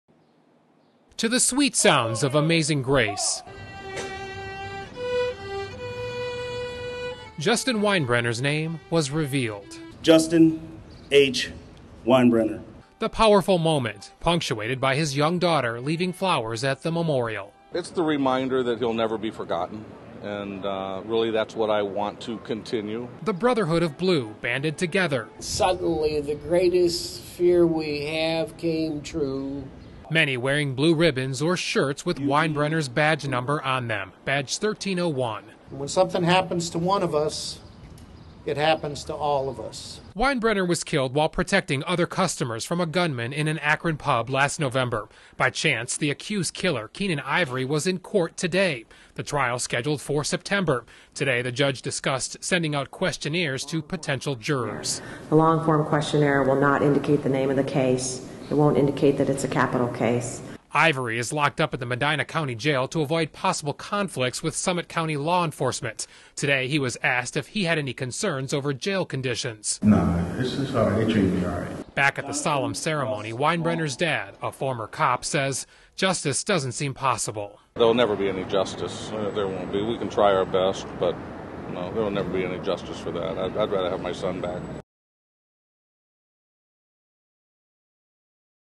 Winebrenner Memorial
Justin Winebrenner added to the Akron Fallen Officer Memorial.